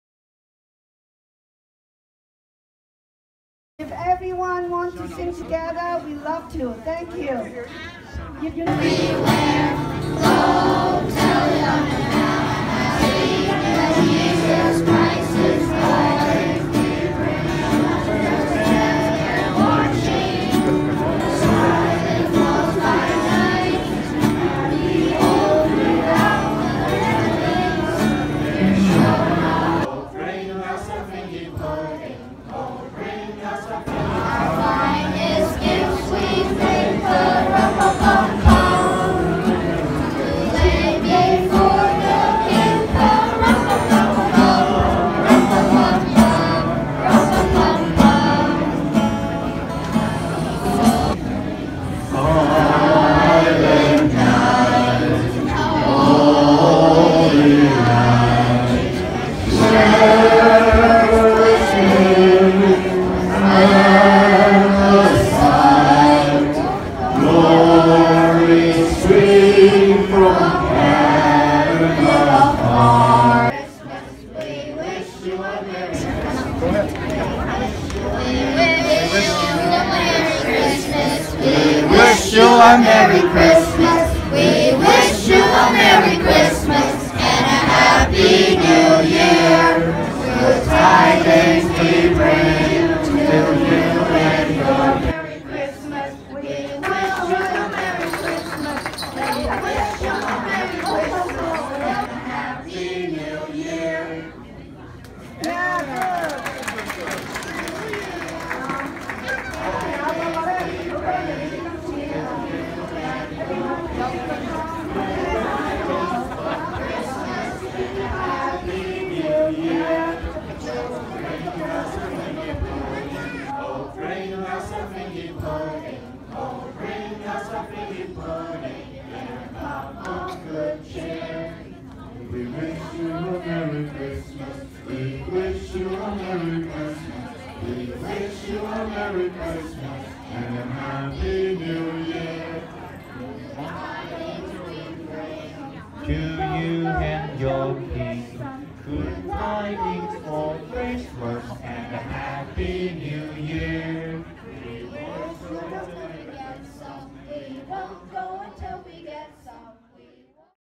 Christmas Dec 25 2017 at St. James Church
Festivities in lower Church after Christmas Day Mass. rd grader CCD class welcome Christmas with Christmas Carols, as Faithful looked forward to the Festivities